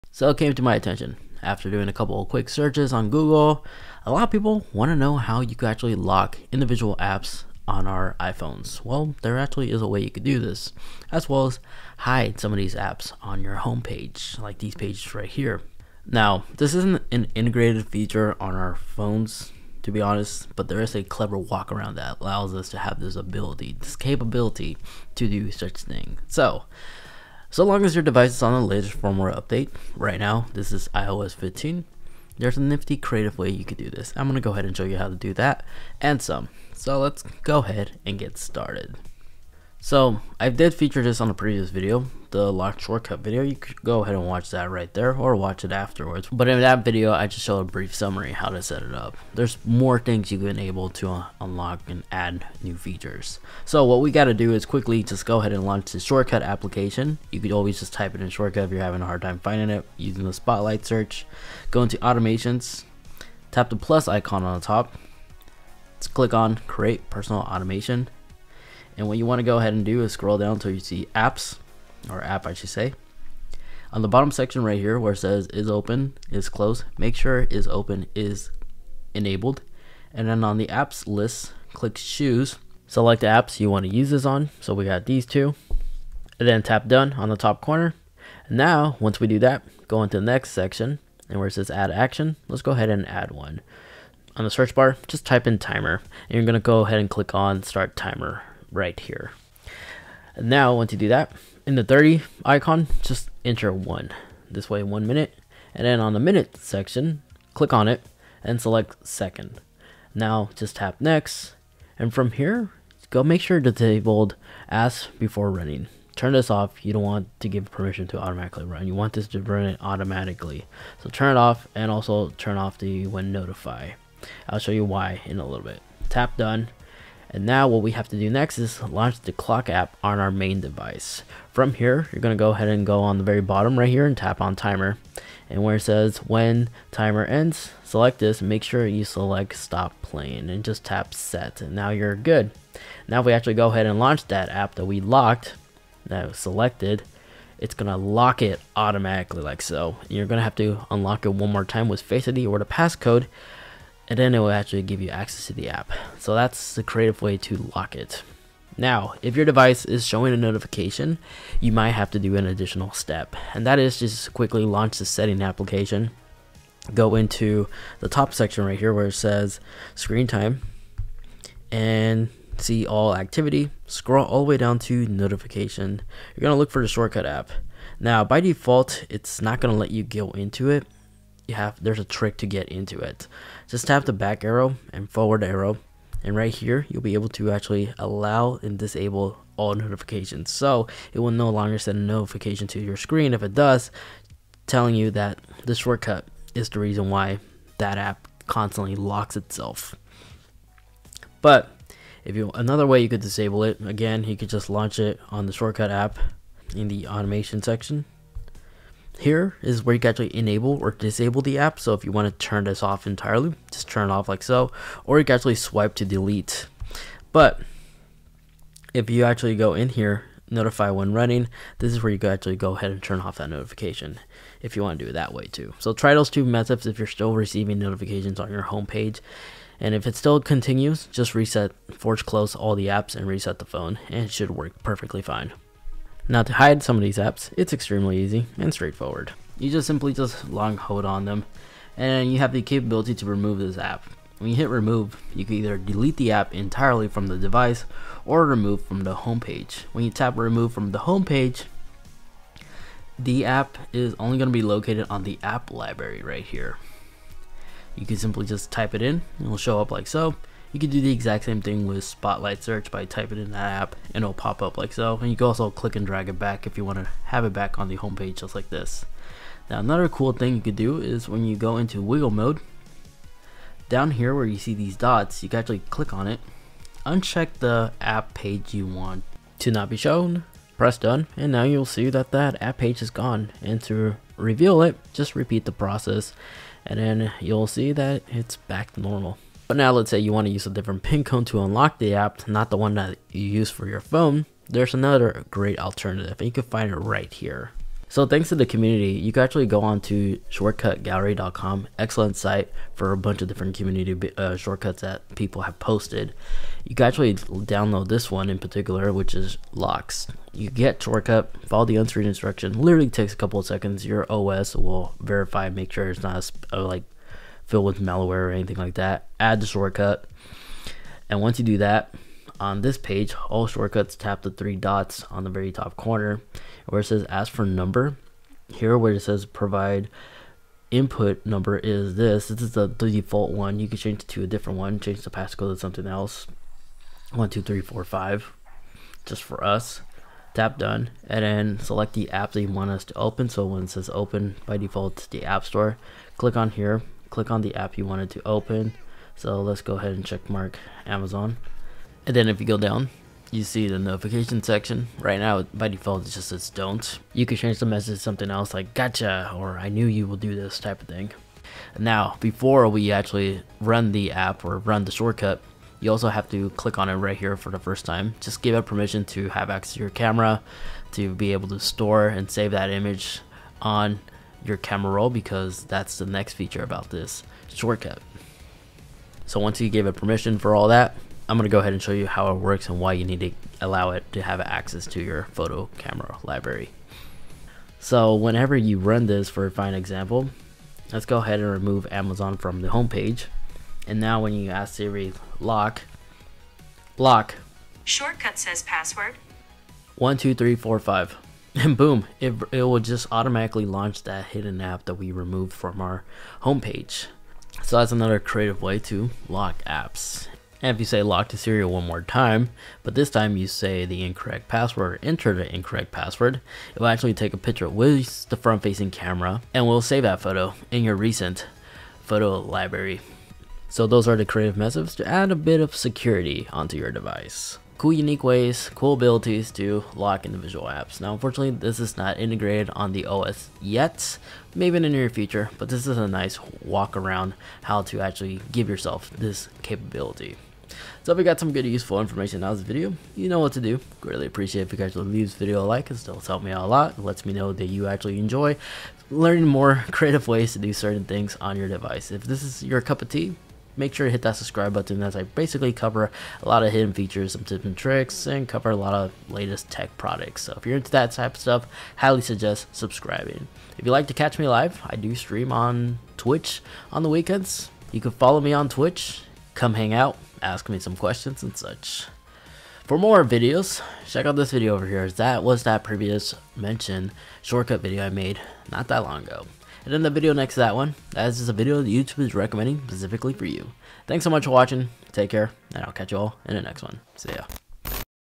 HOW to LOCK & HIDDE Apps on iPhones!
►Timestamps & links
00:00 - Intro
00:54 - Shortcut set up
02:30 - Turn off notifaction and settings
03:56 - Hidde apps and pages
04:50 - Lock shortcut

Picture Fames on the wall by Displate -